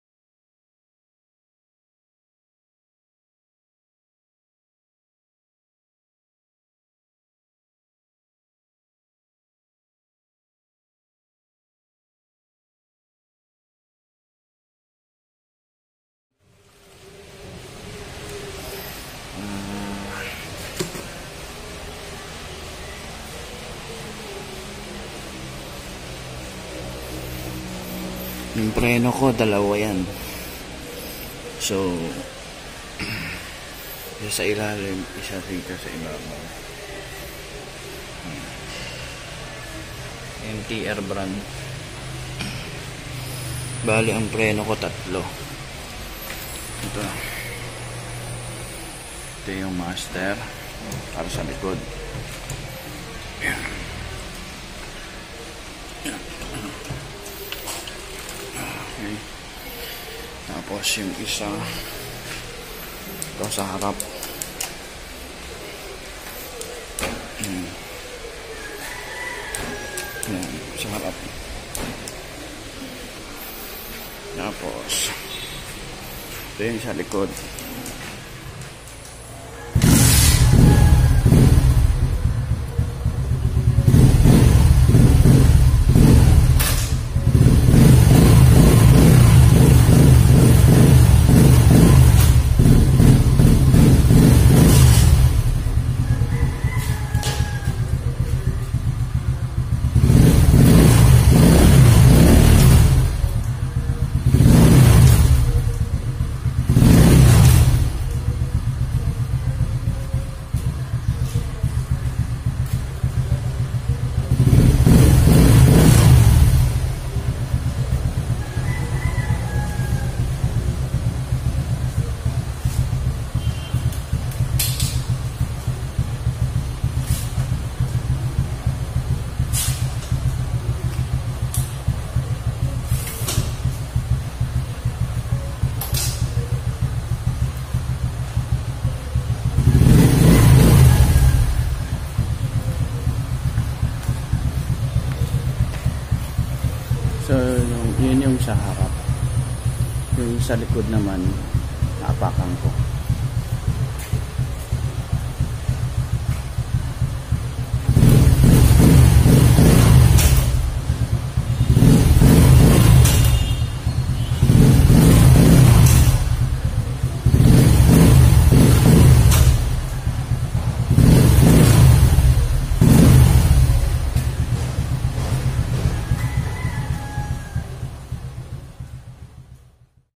Motorcycle Disc Brakes Conversion
3 Brakes by MTR Brand Dual Piston, Rear brakes Dual Caliper.